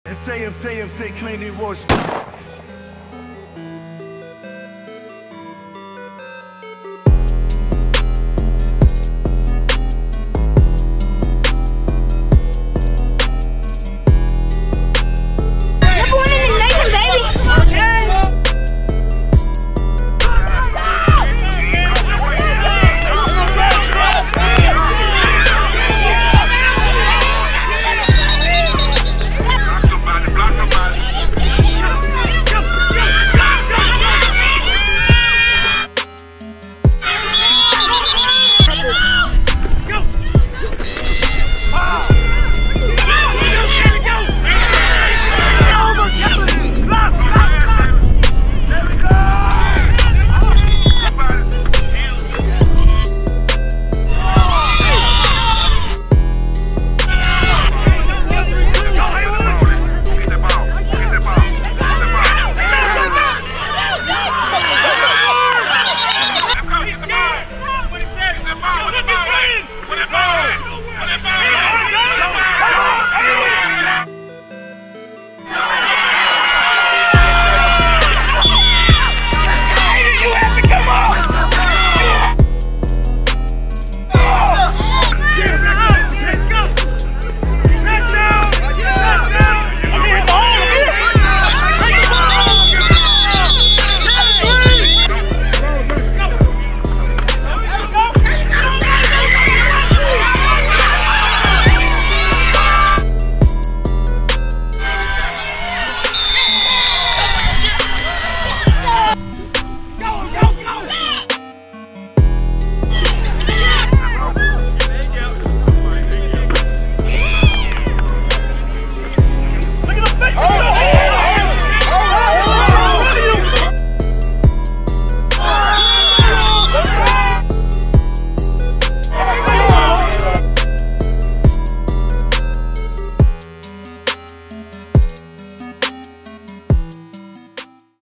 The MOB!!! | 7U Gorilla Mob 🦍 vs 7U RCU 🐎 | HTX Warzone
7U ABN aka Gorilla Mobb formerly known as the CRU plays in their RIVALRY game in College Station, TEXAS.